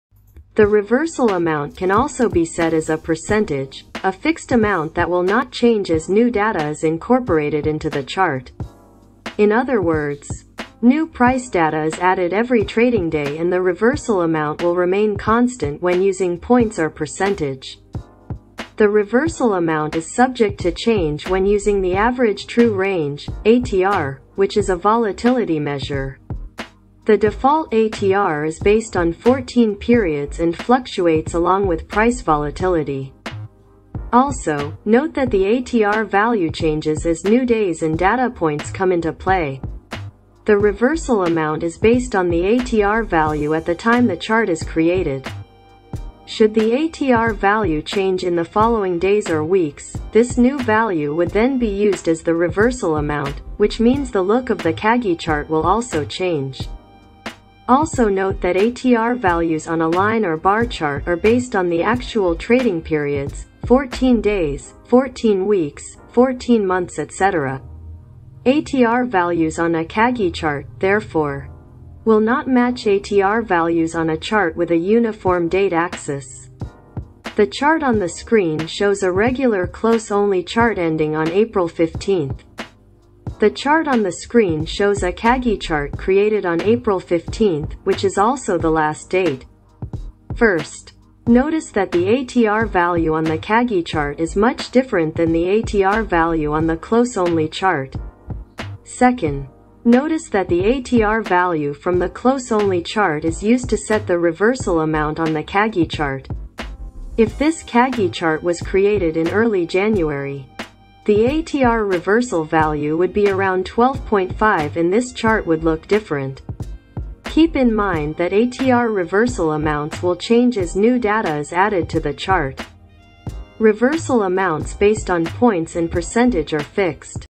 Should we use Fixed Amount or ATR in Kagi Chart ?
The reversal amount can also be set as a percentage, a fixed amount that will not change as new data is incorporated into the chart.

In other words, new price data is added every trading day and the reversal amount will remain constant when using points or percentage.

The reversal amount is subject to change when using the Average True Range (ATR), which is a volatility measure.

The default ATR is based on 14 periods and fluctuates along with price volatility.

Trading Course